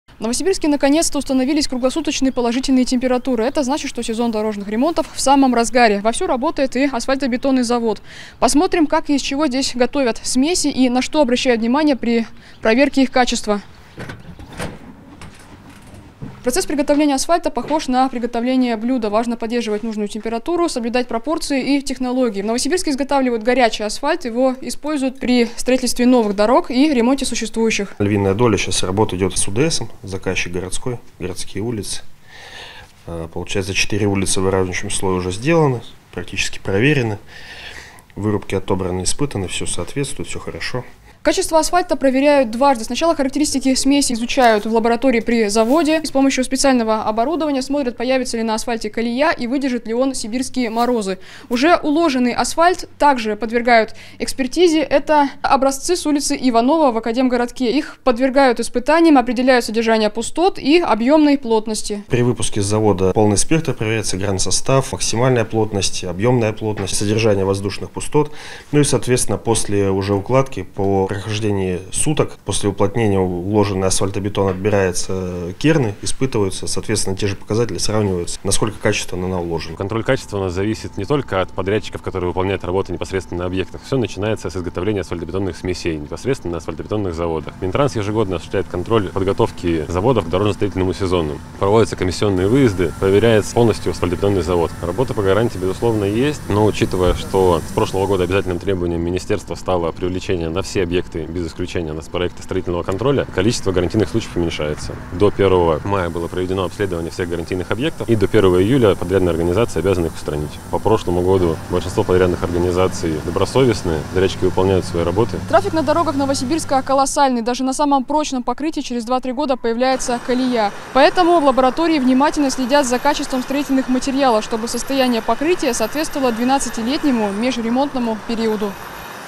Новый асфальт в Новосибирске проверяют на стойкость к износу и морозам
Качество нового асфальта в Новосибирске проверяют в специальной лаборатории. Оценивают устойчивость к перепадам температур, вероятность образования колеи и износостойкость.